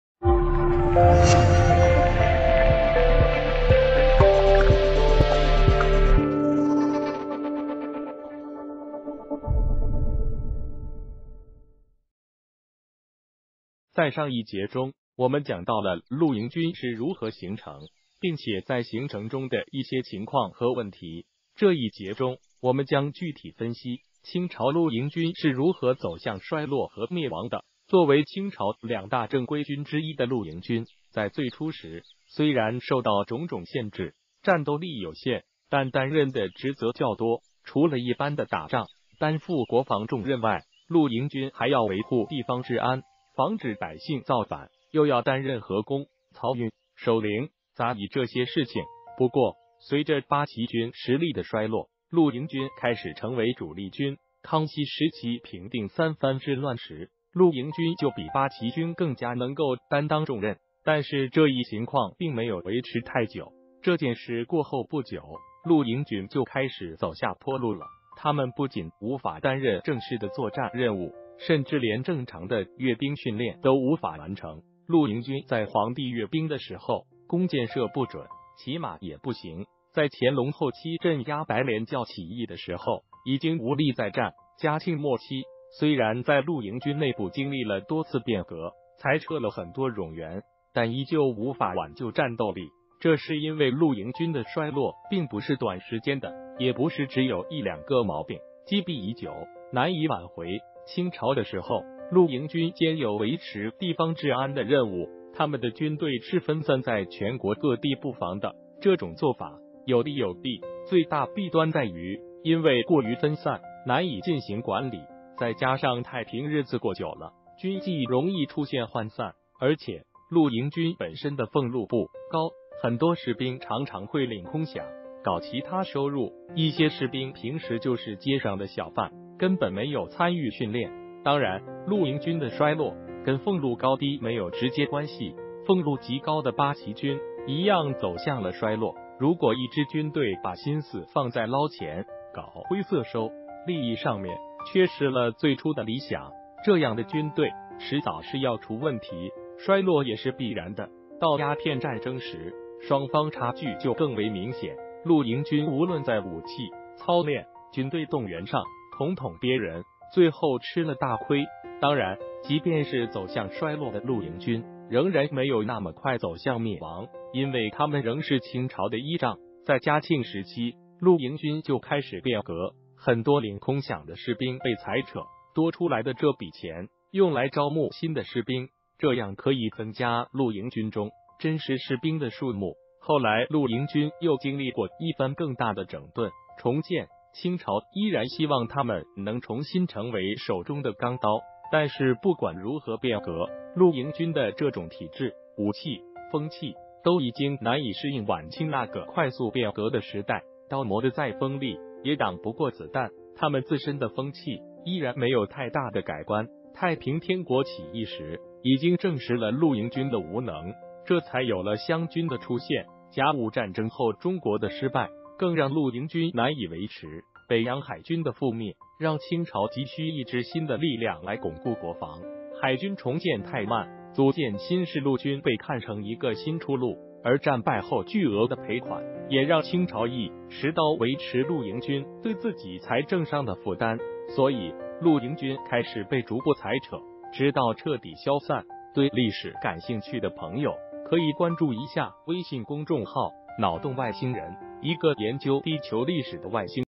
清朝绿营军如何走向衰落和灭亡？
谢谢！清朝绿营军如何走向衰落和灭亡？